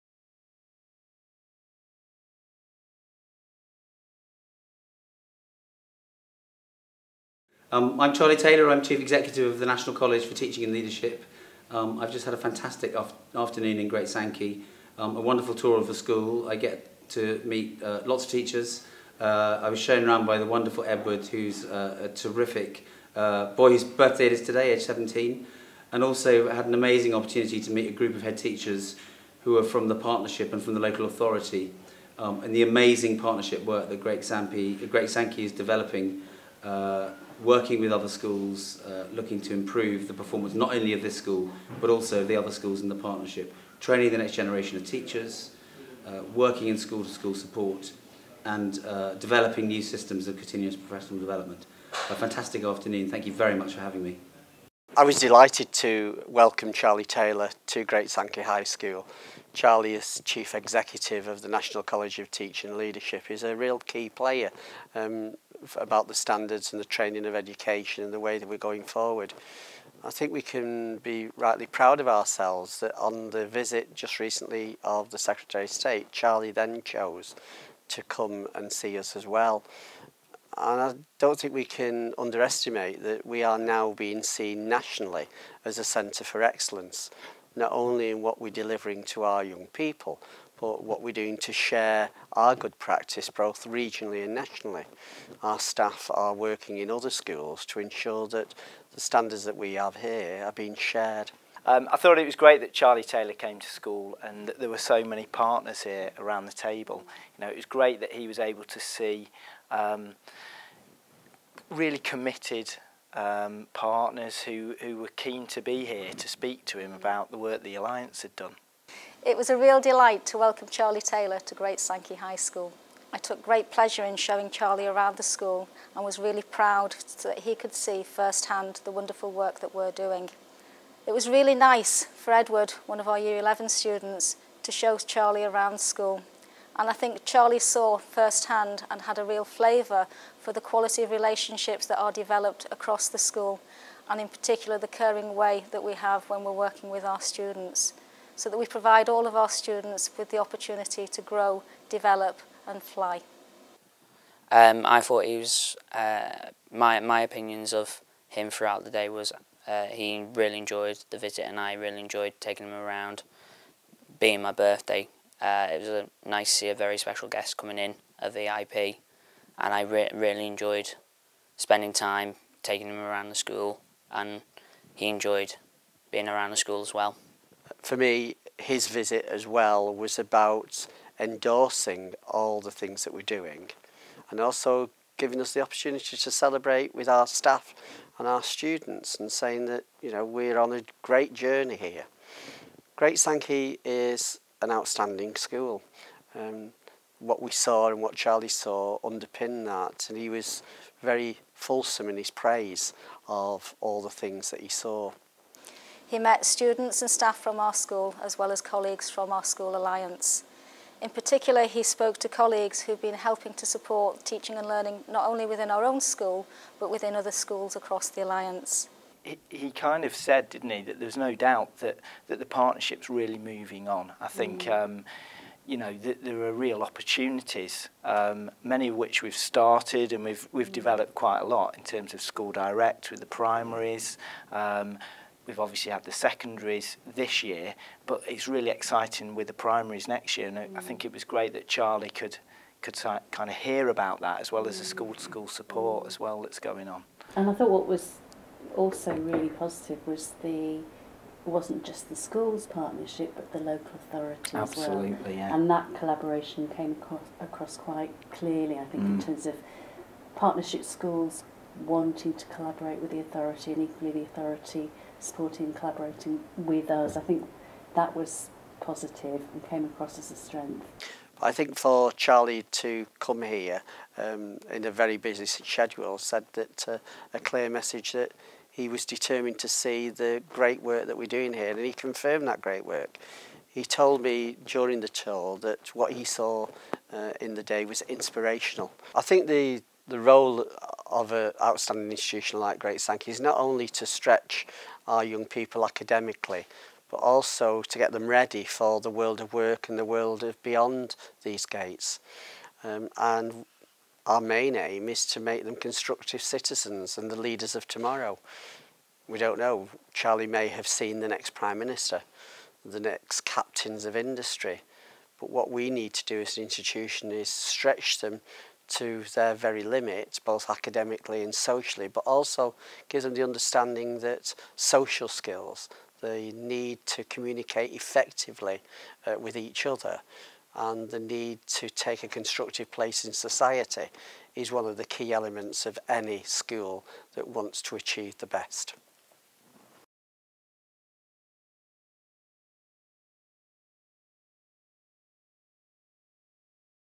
Charlie Taylor Visit 2014
Visit by Charlie Taylor, Chief Executive of the National College of Teaching and Leadership to Great Sankey High School.